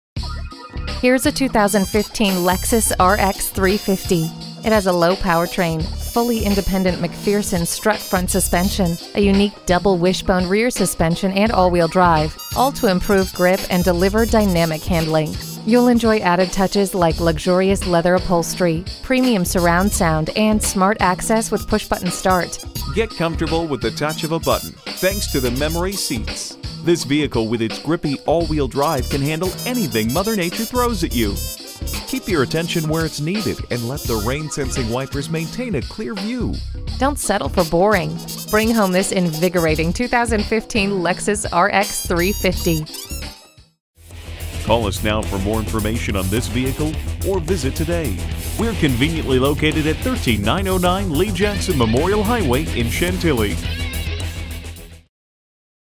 New 2015 Lexus RX 350 Chantilly VA Washington-DC, MD RXFC55424 - SOLD
Year: 2015
Make: Lexus
Model: RX 350
Trim: AWD 4dr
Engine: L 6 Cyl.
Color: Obsidian
Interior: LC20
Stock #: RXFC55424

Address:
13909 Lee Jackson Memorial Hwy